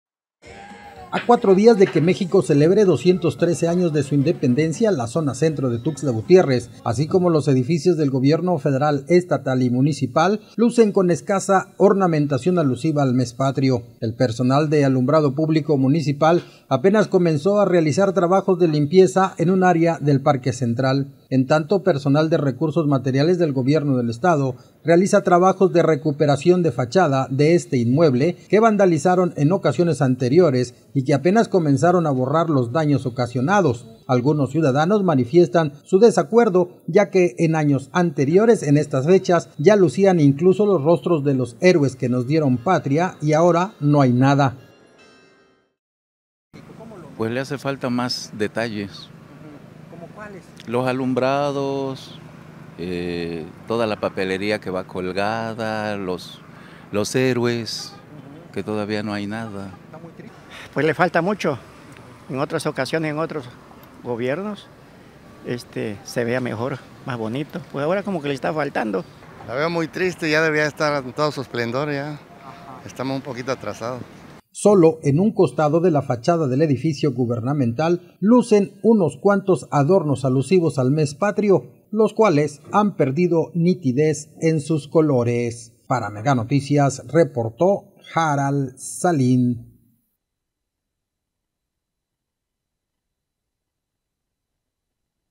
Plaza central de la capital chiapaneca con escasa ornamentación alusiva al mes patrio
Escasa ornamentación alusiva a fiestas patrias en la capital de Chiapas